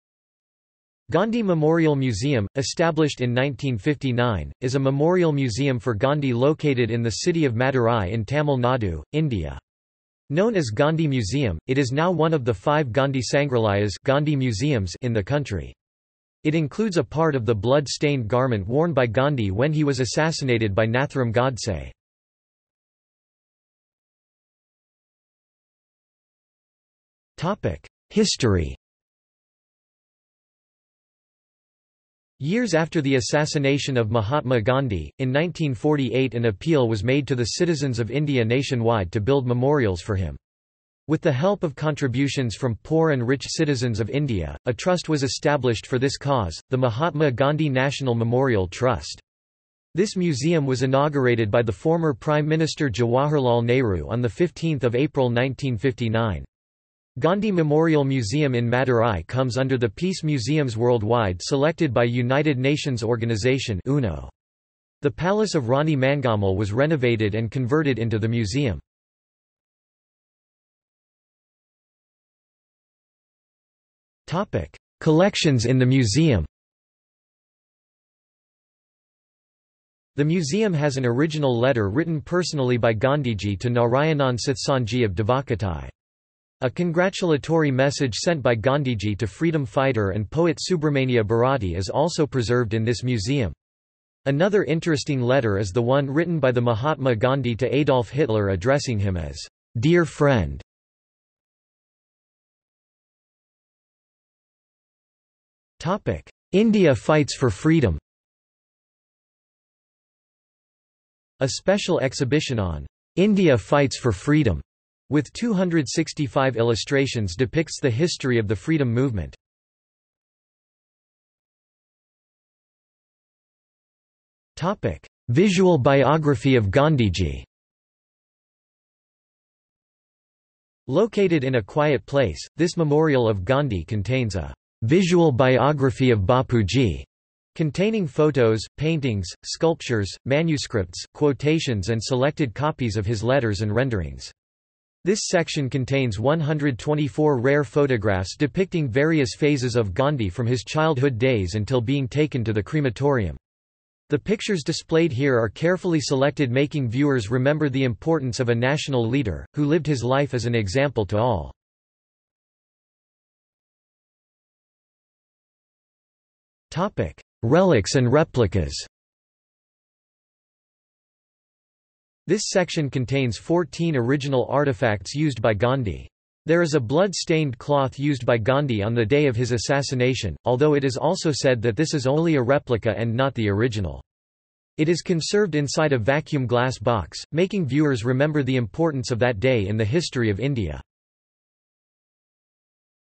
Gandhi Memorial Museum, Madurai | Wikipedia audio article
This is an audio version of the Wikipedia Article:
Gandhi Memorial Museum, Madurai

00:00:23 1 History
00:01:10 2 Collections in the museum
00:01:42 2.1 India Fights for Freedom
00:02:01 2.2 Visual Biography of Gandhiji
00:02:43 2.3 Relics and Replicas

- Socrates

SUMMARY
Gandhi Memorial Museum, established in 1959, is a memorial museum for Gandhi located in the city of Madurai in Tamil Nadu, India. Known as Gandhi Museum, it is now one of the five Gandhi Sanghralayas (Gandhi Museums) in the country.  It includes a part of the blood-stained garment worn by Gandhi when he was assassinated by Nathuram Godse.